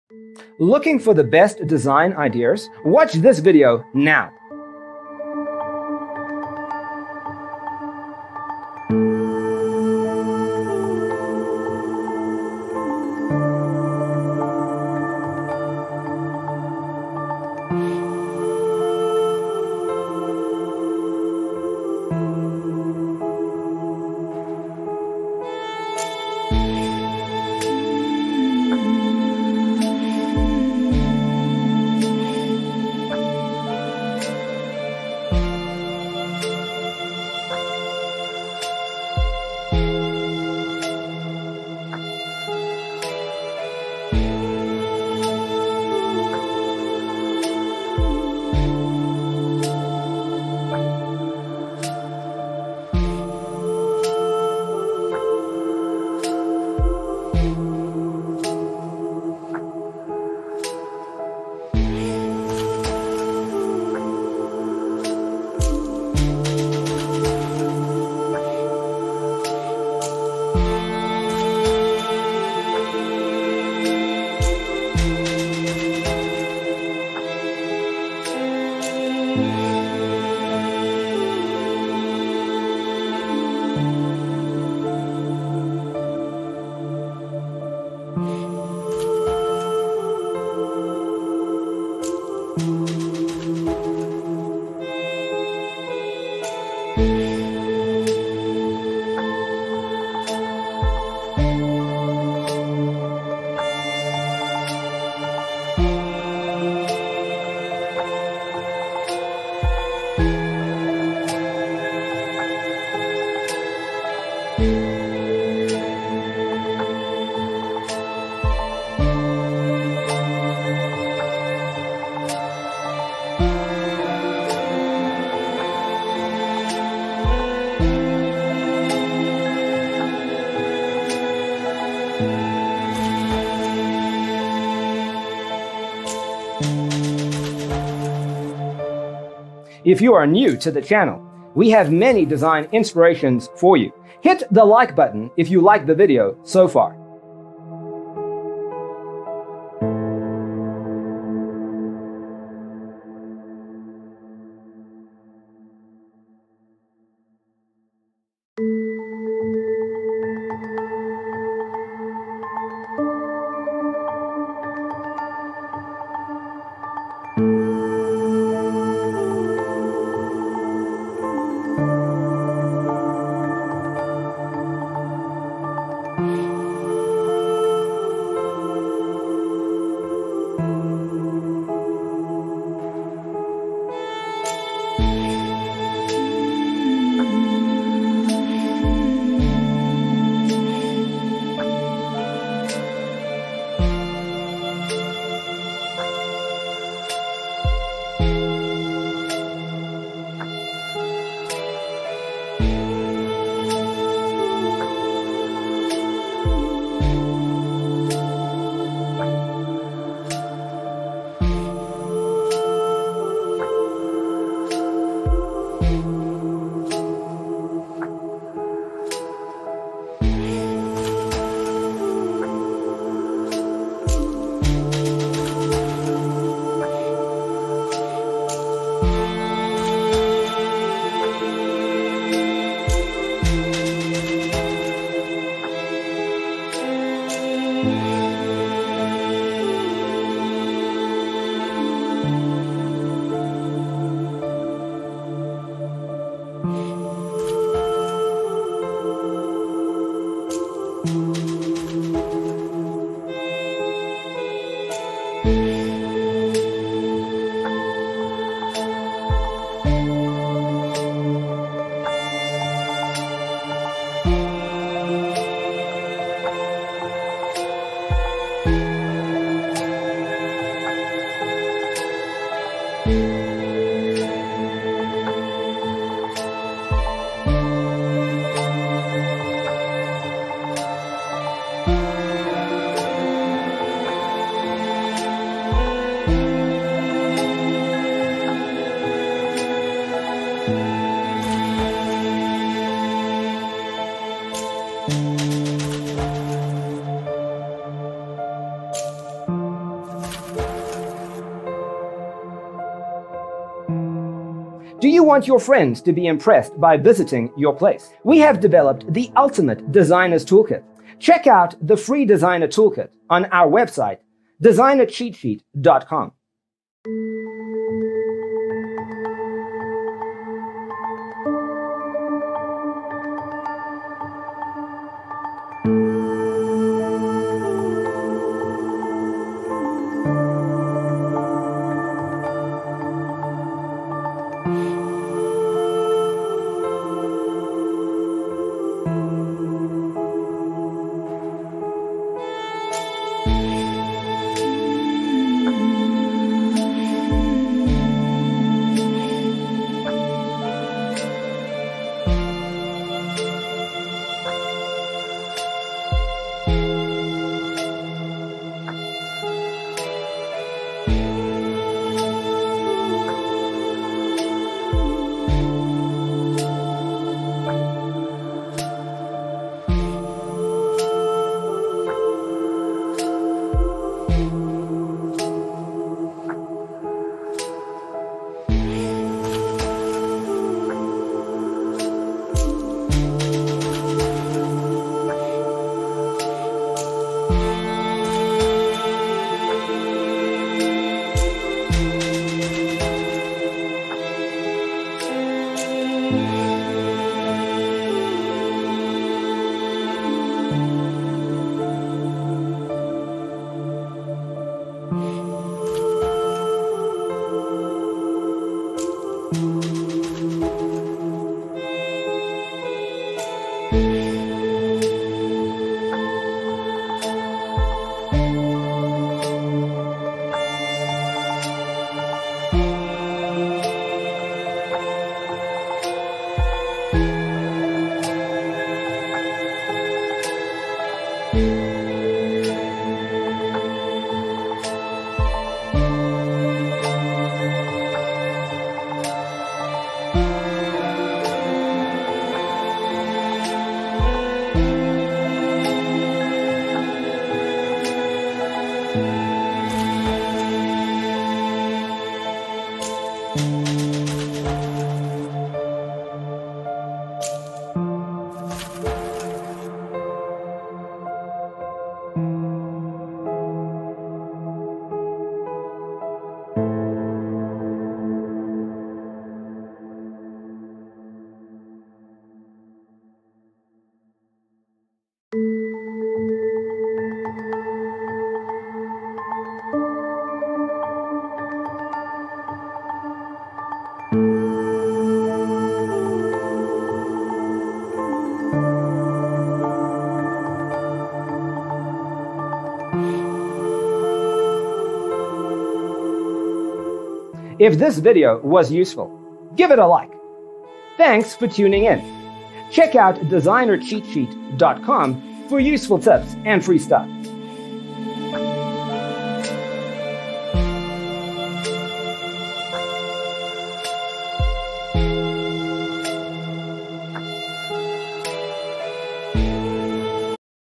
75 Modern White Living Room Design Ideas You'll Love ⭐
Browse modern white living room decorating ideas and furniture layouts. Discover design inspiration from a variety of modern white living rooms, including color, decor and storage options.

0:00:00 - Intro
0:00:12 - Modern White Living Room Design Ideas You'll Love: Inspirations 1 to 25
0:03:03 - Modern White Living Room Design Ideas You'll Love: Inspirations 26 to 50
0:06:07 - Modern White Living Room Design Ideas You'll Love: Inspirations 51 to 75

Hey hello!